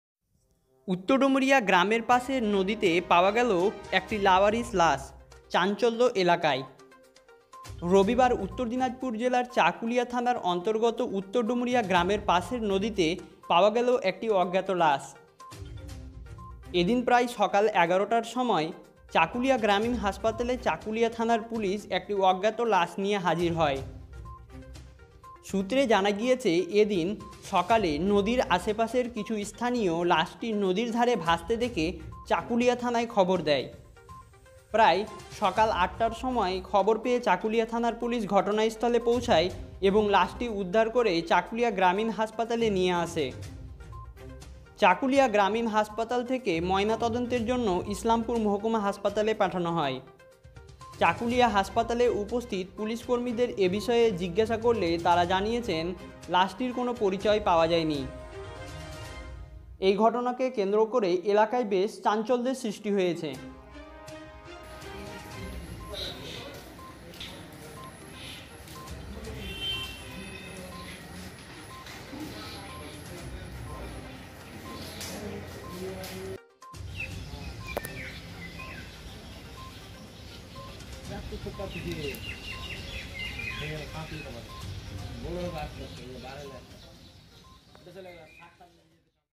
উত্তর ডুমুরিয়া গ্রামের পাশে নদীতে পাওয়া গেল একটি লাওয়ারিশ লাশ, চাঞ্চল্য এলাকায়।
রবিবার উত্তর দিনাজপুর জেলার চাকুলিয়া থানার অন্তর্গত উত্তর ডুমুরিয়া গ্রামের পাশে নদীতে পাওয়া গেল একটি অজ্ঞাত লাশ।

এদিন প্রাইস সকাল 11 টার সময় চাকুলিয়া গ্রামীণ হাসপাতাল চাকুলিয়া থানার পুলিশ একটি অজ্ঞাত লাশ নিয়ে হাজির হয়।

সূত্রে জানা গিয়েছে এরিন সকালে নদীর আশেপাশের কিছুই স্থানীয় লাশটি নদীর ধারে ভাসতে দেখে চাকুলিয়া থানায় খবর দেয়।

প্রায় সকাল আটটার সময়
খবর পেয়ে চাকুলিয়া থানার পুলিশ ঘটনাস্থলে পৌঁছায় এবং লাশটি উদ্ধার করে চাকুলিয়া গ্রামীণ হাসপাতালে নিয়ে আসে। এবং চাকুলিয়া গ্রামীণ হাসপাতাল থেকে ময়নাতদন্তের জন্য ইসলামপুর মহকুমা হাসপাতালে পাঠানো হয়।

চাকুলিয়া হাসপাতালে উপস্থিত পুলিশকর্মীদের এ বিষয়ে জিজ্ঞাসা করলে তারা জানায় লাশটির পরিচয় এখনো পাওয়া যায়নি।

এই ঘটনাকে কেন্দ্র করে এলাকায় বেশ চাঞ্চল্যের সৃষ্টি হয়েছে।